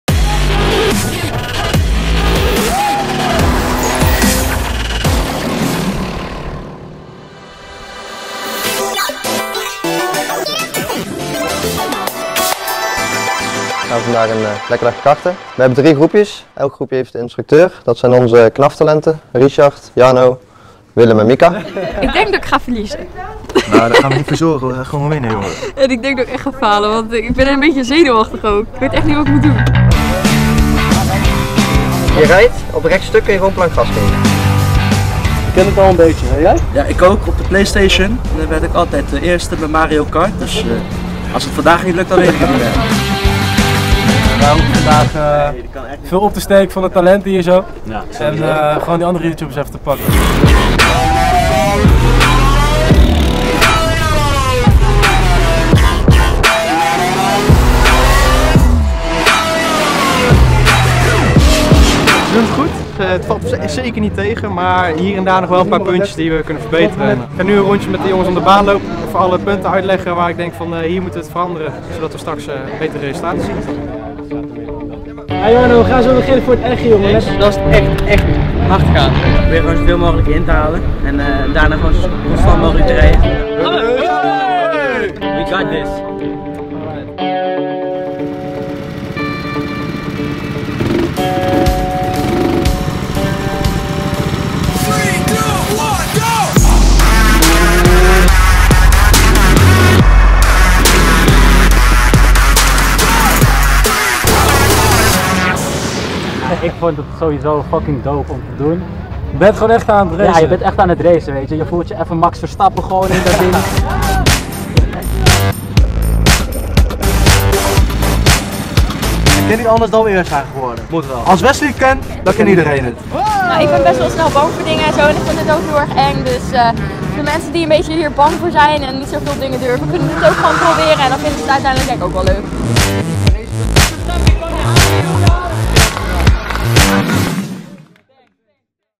IEDEREEN KAN RACEN
Iedereen kan racen! Daarom nodigde de KNAF de vloggers van Ontycholijk, thingsjulialoves, Zoufox en Gewoon Doen uit voor een kartbattle tegen autosporttalenten Richard Verschoor, Jarno Opmeer en Willem Meijer!